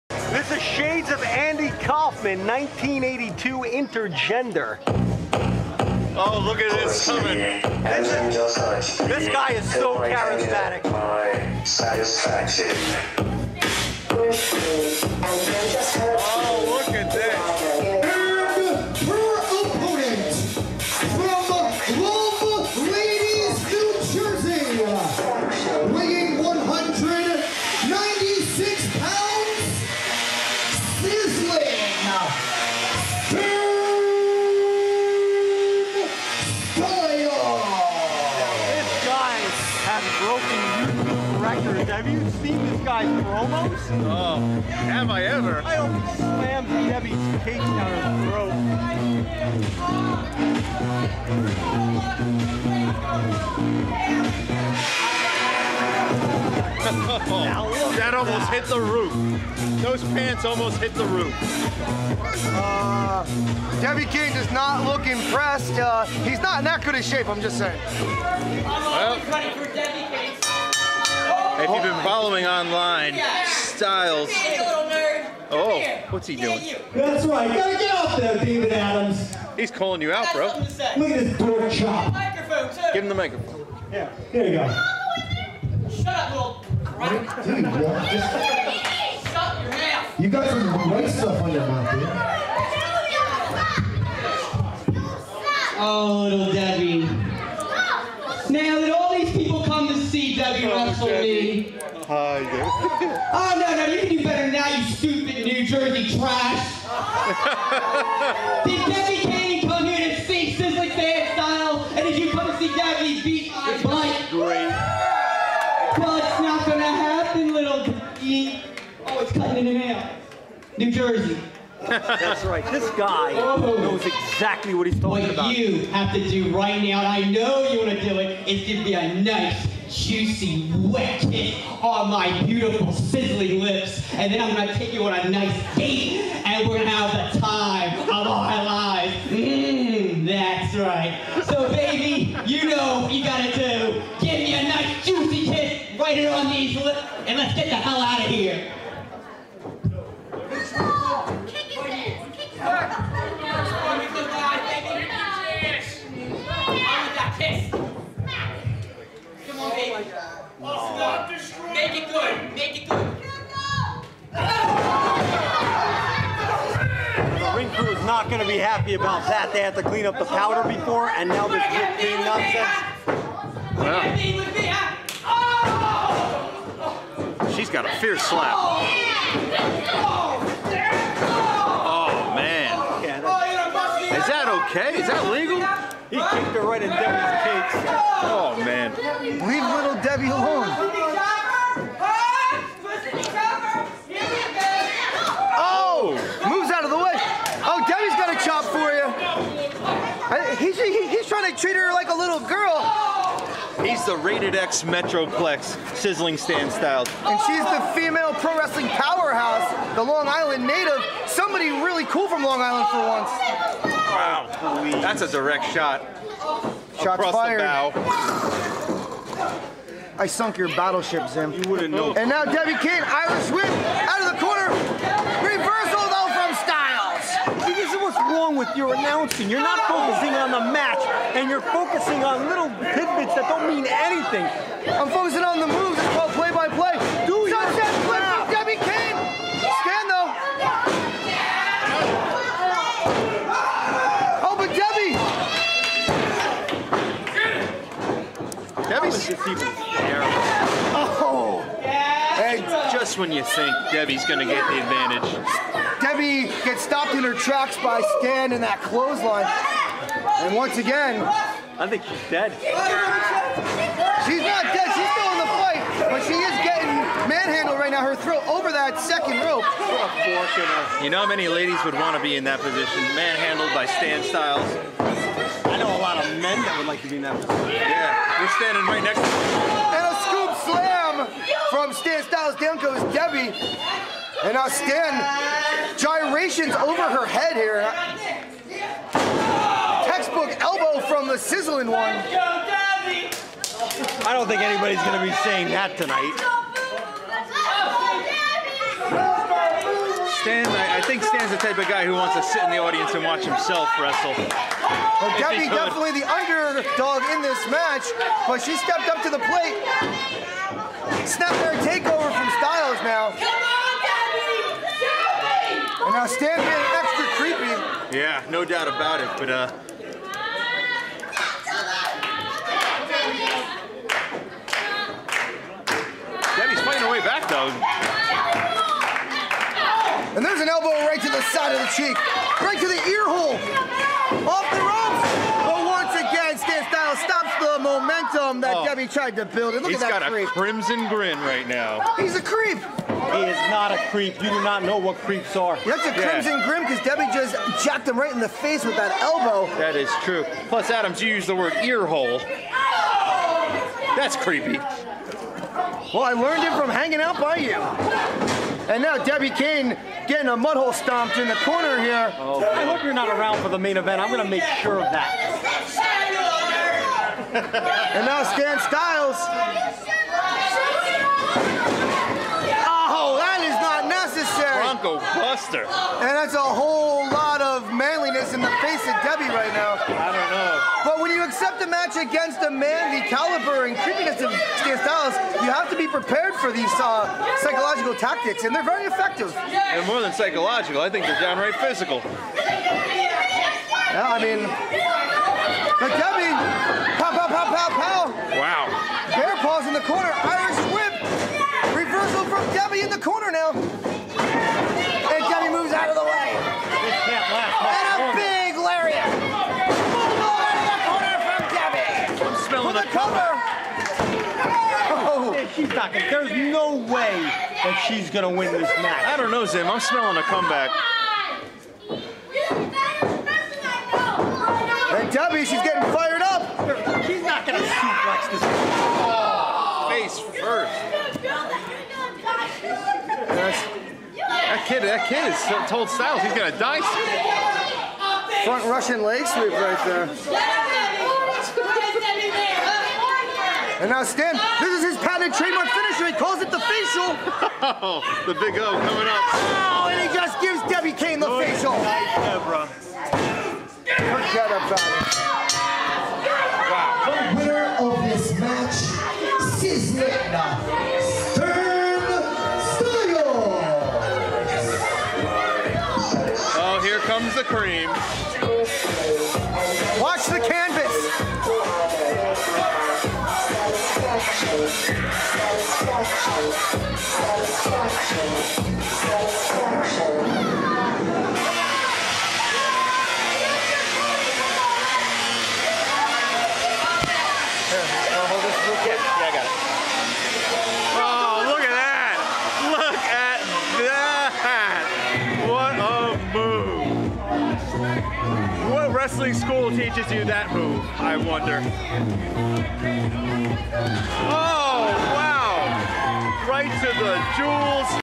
Sizzling Stan Stylez vs Debbie Kane BBWF 6-10-16
BBWF 6-10-16 One Night Only Inter-gender Match Sizzling Stan Styles vs Deborah Kane

Follow @thesizzlingone
Facebook.com/realstanstyles